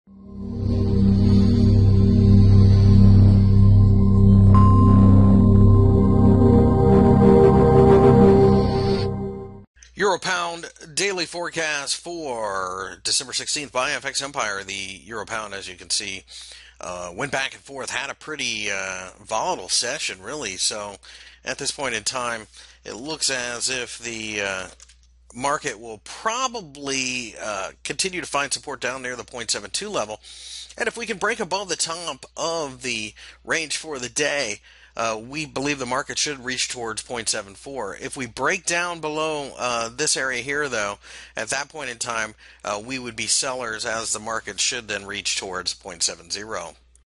EUR/GBP Technical Analysis for December 16 2015 by FXEmpire.com
- December 16 2015 currency daily technical analysis for the EUR/GBP pair. Forex Technical Analysis Forex Fundamental Analysis and Forex Brokers on FX Empire